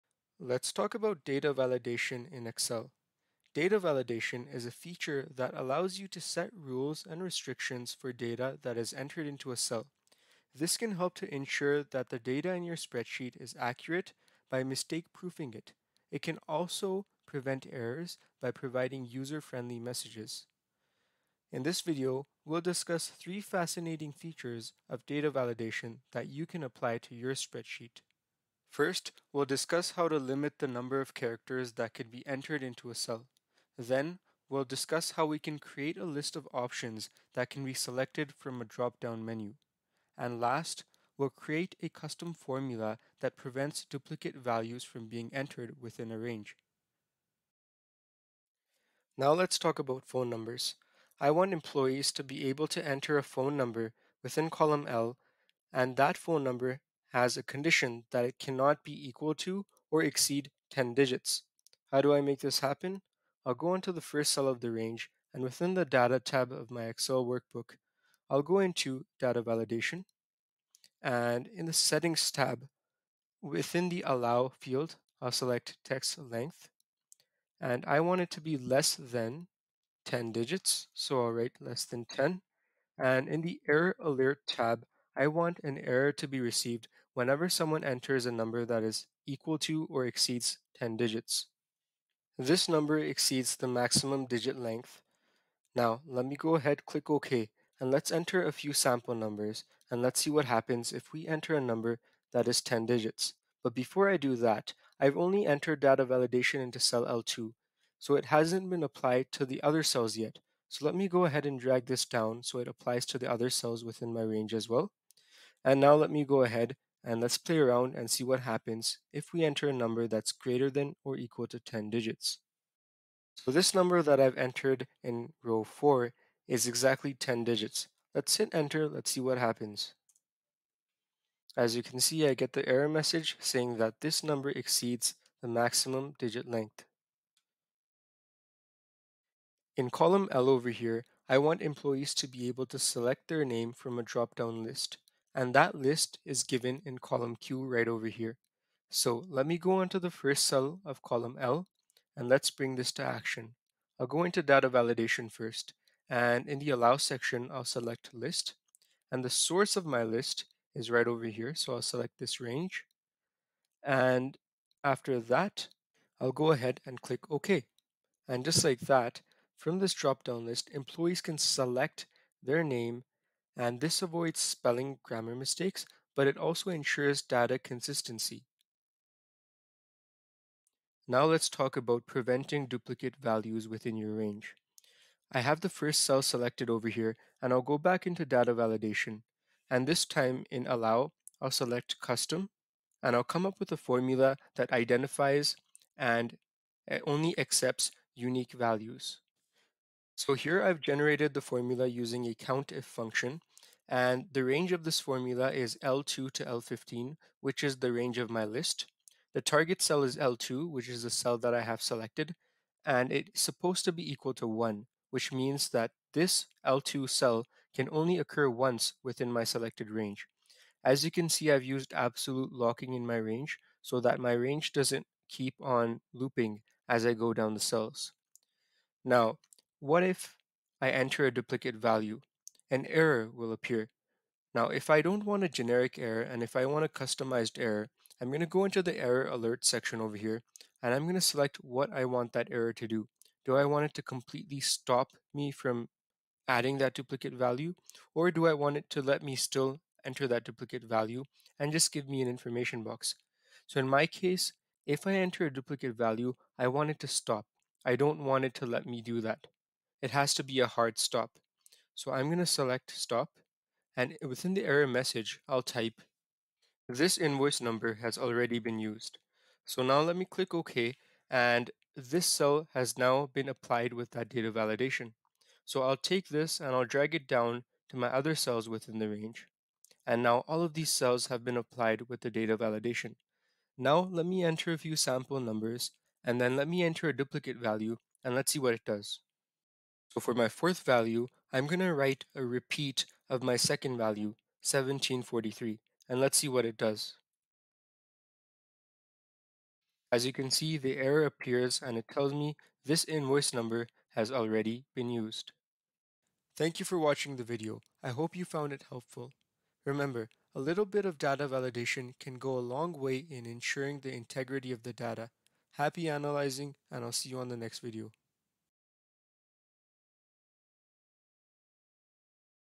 Master Data Validation in Excel - Unlock the Magic!
In this tutorial, we dive into the world of data validation in Excel. Data validation is a powerful tool that can help ensure the integrity of your data by setting rules and constraints on the data that is entered into your spreadsheet. We'll show you how to use data validation to set up rules for cell values, create drop-down lists, and even customize error messages. Whether you're a beginner or an advanced user, this tutorial will provide you with the knowledge and skills you need to take your data analysis to the next level. So, join us as we explore the ins and outs of data validation in Excel and learn how to make your data work for you!